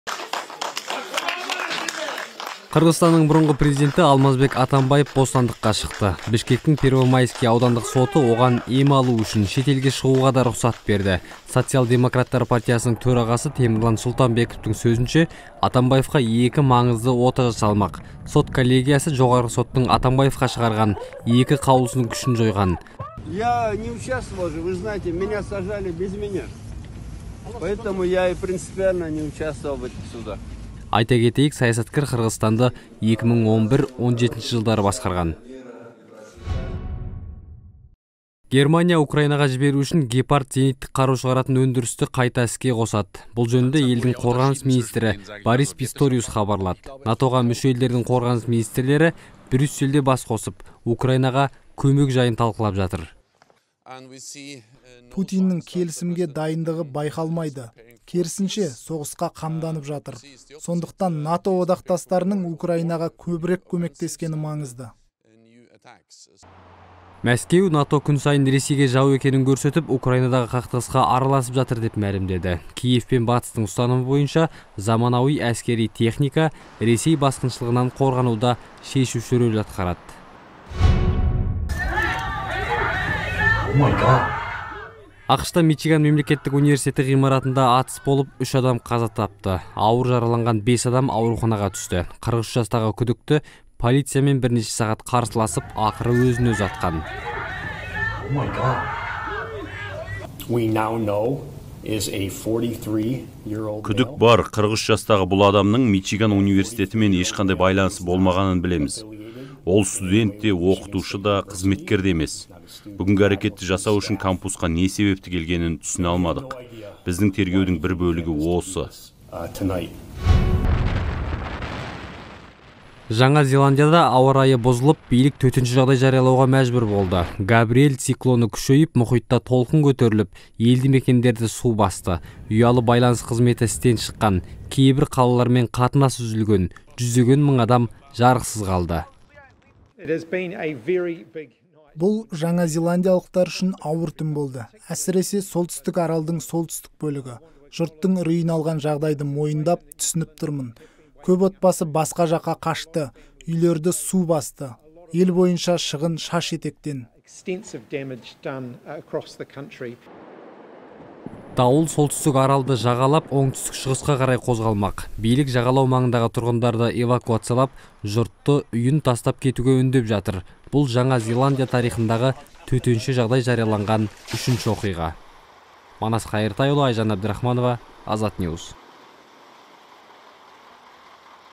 Атамбаевтың бостандығы, НАТО-ның қуаты, АҚШ-тағы атыс - Жаһан жаңалықтары
Қырғызстанның бұрынғы президенті Алмазбек Атамбаев түрмеден шықты. Ол ешқандай кінәсі жоғын, енді шетелге барып емделетінін айтты. Ал Брюссельде НАТО елдерінің қорғаныс министрлері кездесіп жатыр. Әлемде 14 ақпанда болған оқиғаларды Азаттық шолуынан көріңіз.